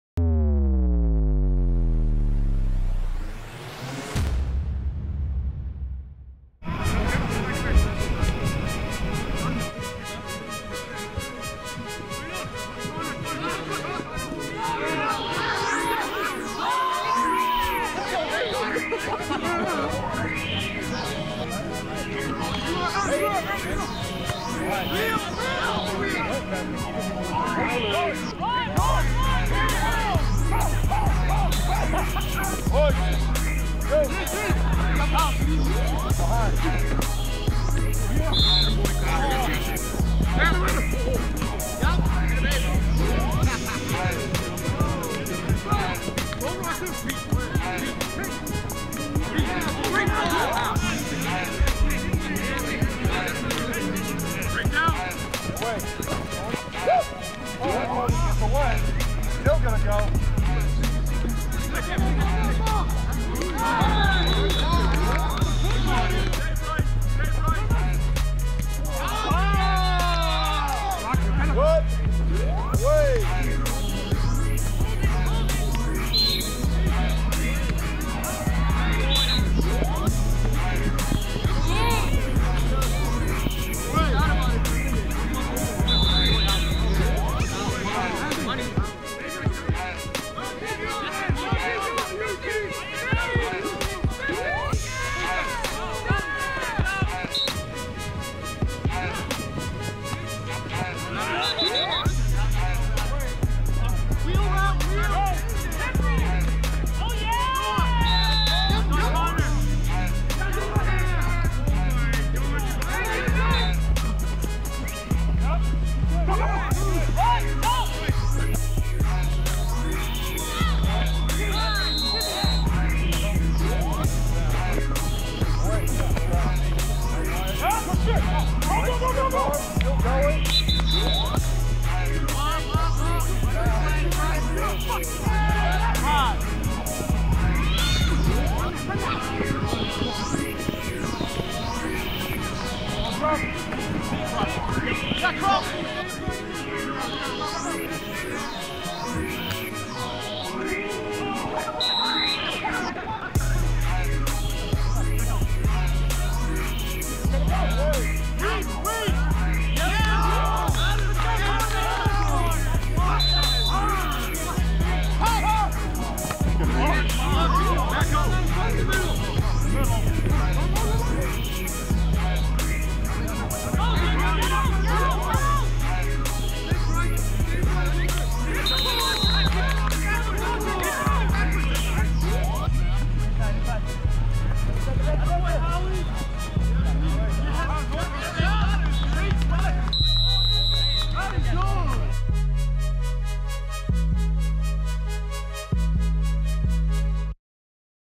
Fundamental Flag Football League | FTB vs 978 Glory
Fundamental Flag Football League 2023 Week 4, Flip The Bird vs 978 Glory Highlight